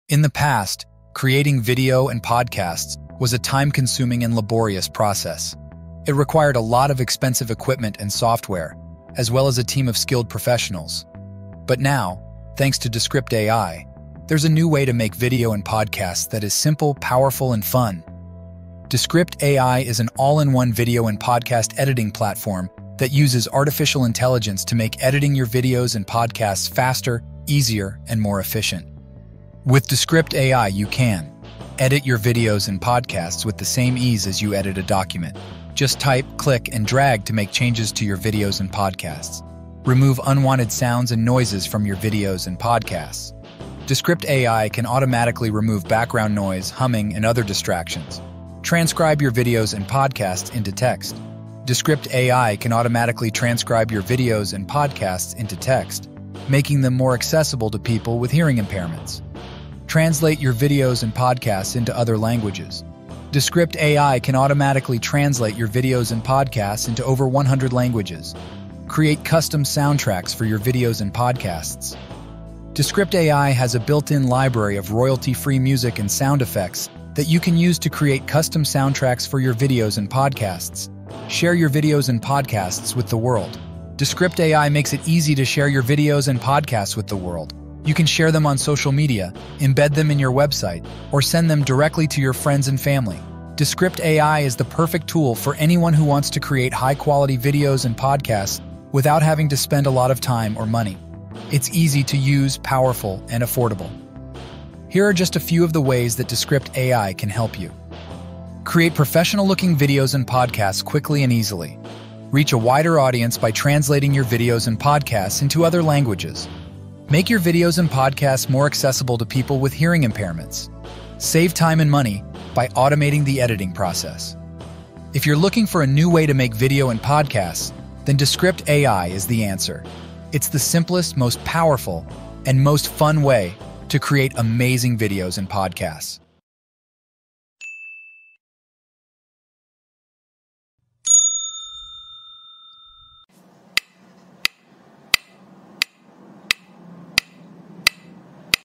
Create professional-looking videos and podcasts quickly and easily.
Looking for a way to create professional-looking videos and podcasts without spending a fortune or spending hours learning how to use complex software? If so, Descript AI is the tool for you.

Descript AI is an AI-powered video and podcast editing platform that simplifies and automates the entire editing process.